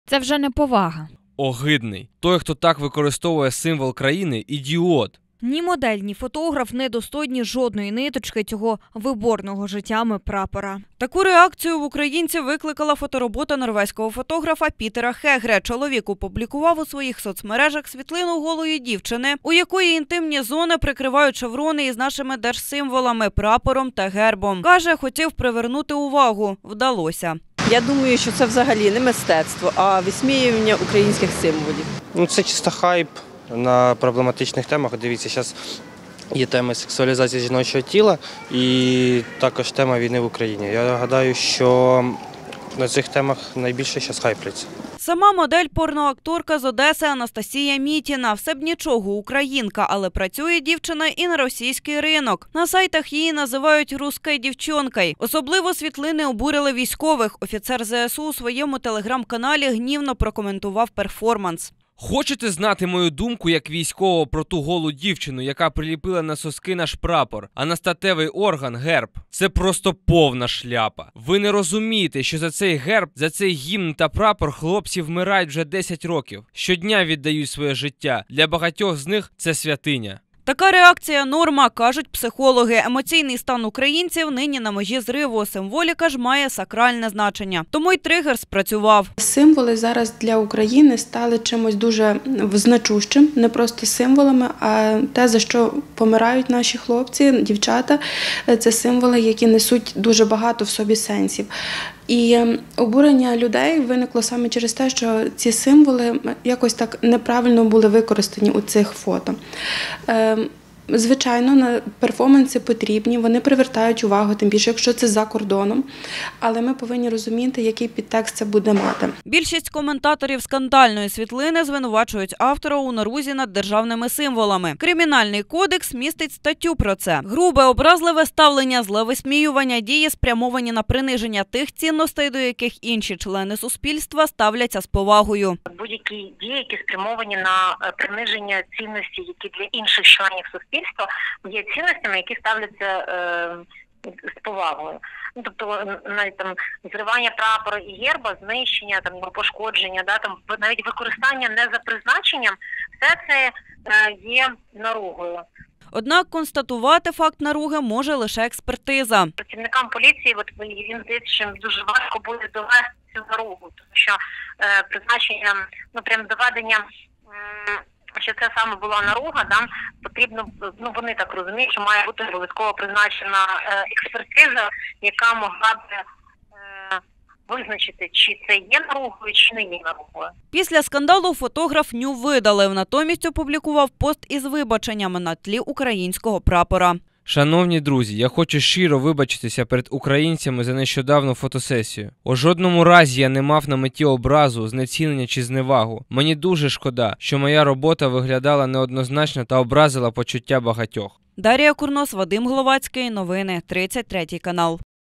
Мистецтво чи нігілізм? Обурення через голі фото з українською символікою на інтимних зонах
Нещодавно у мережі спалахнув скандал через роботу норвезького фотографа Пітера Хегре. Чоловік зафільмував жінку у стилі ню. Інтимні частини тіла, прикриті українським прапором та гербом. За його словами, хотів привернути увагу аудиторії до проблеми повномасштабного вторгнення росіян у нашу країну. Однак глядачі сприйняли "перформанс" неоднозначно – звинуватили у паплюженні державної символіки.